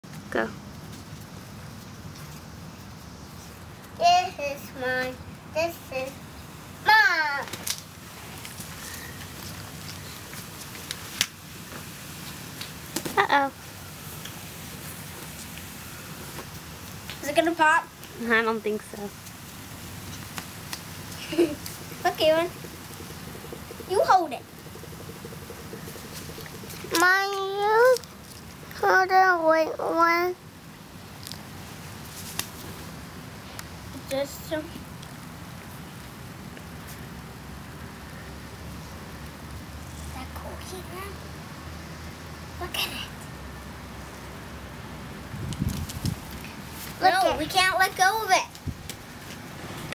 Magic Balloon
Inflate a balloon with these two ingredients.